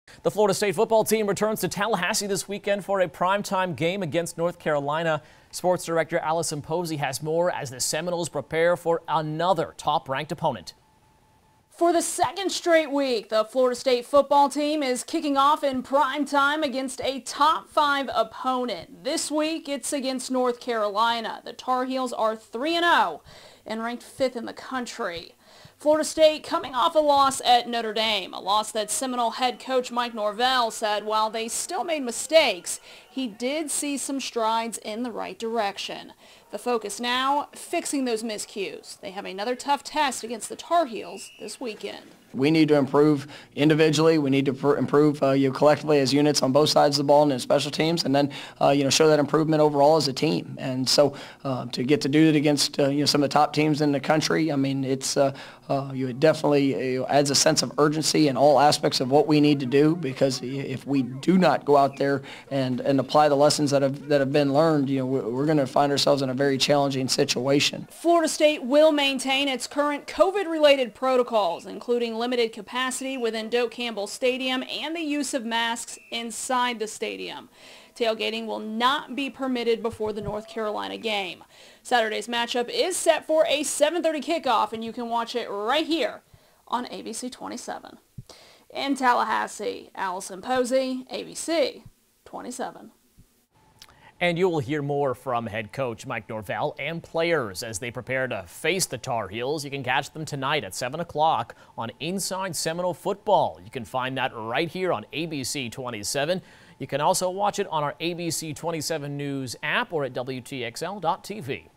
Seminoles face another top-5 program Saturday in North Carolina Tar Heels
For the second straight week, the Florida State football team is kicking off in prime time against a top-five program.